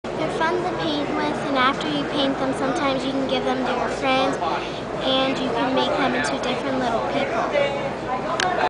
Nicole's Kookle Gift Ideas
Paint a Kookle Nesting Dolls as a present to avoid traffic in the mall!